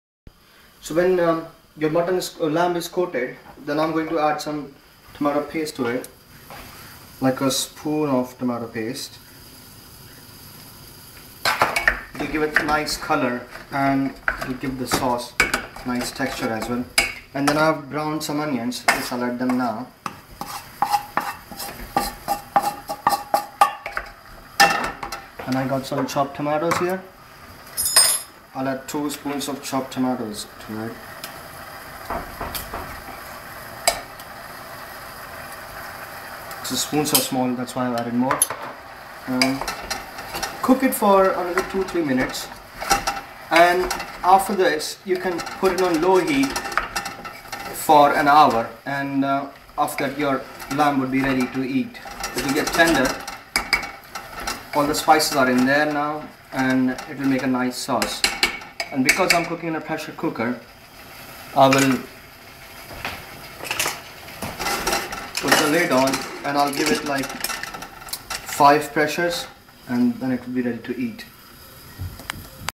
mutton lamb curry part 3
An Indian recipe for mutton lamb curry